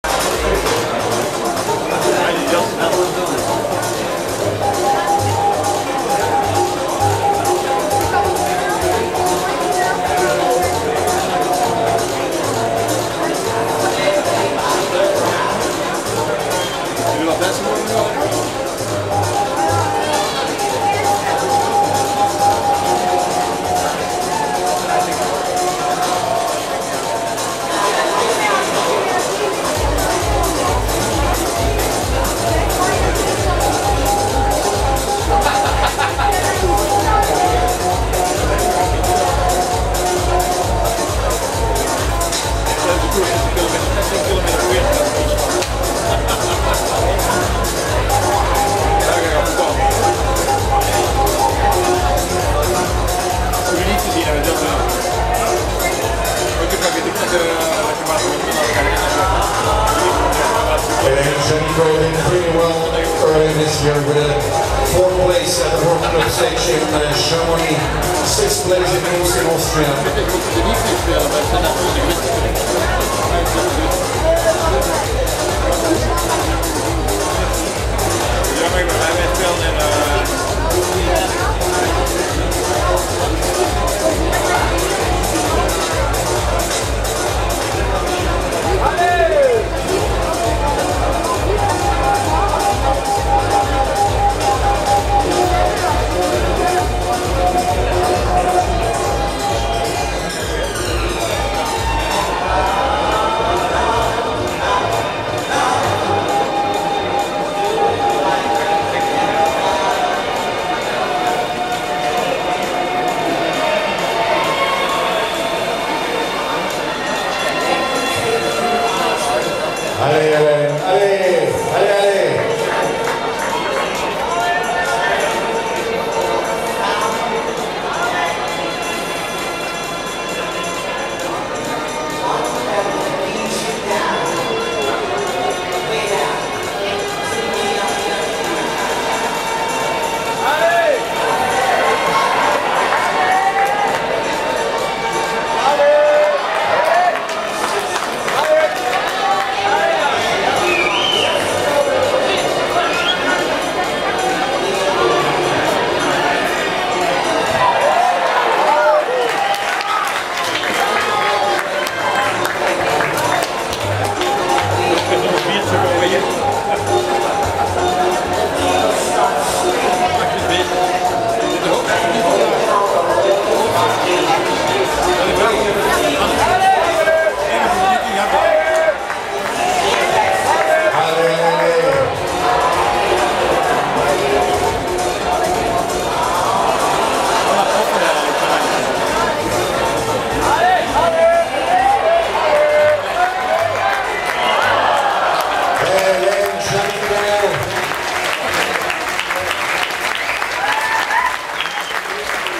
IFSC World Cup Lead - Puurs - 2012 - Finals - Hélène Janicot
Hélène Janicot in the finals of the ifsc lead world cup in Puurs on september 22th 2012